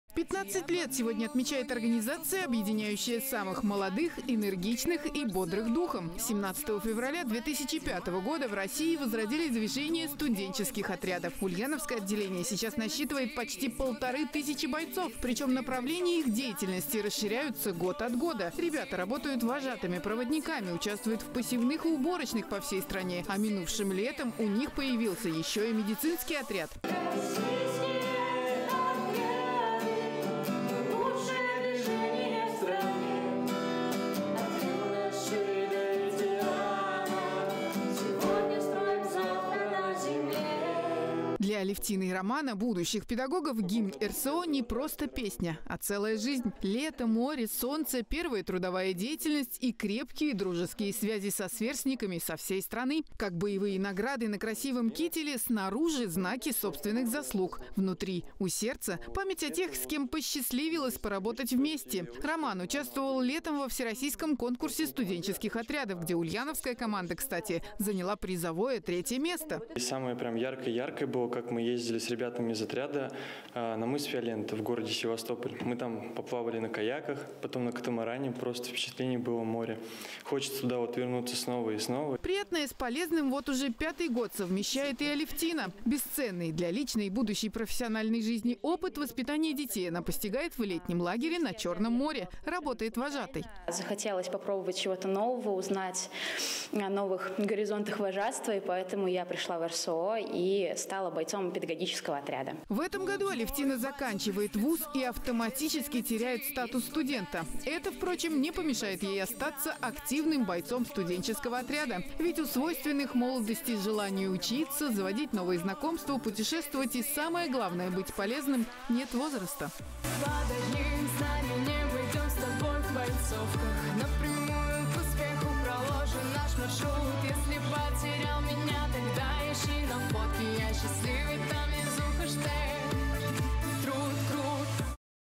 Движение Студенческих отрядов
15 лет сегодня отмечает организация, объединяющая  самых молодых, энергичных и бодрых духом! 17 февраля 2005 года в России возродили движение Студенческих отрядов.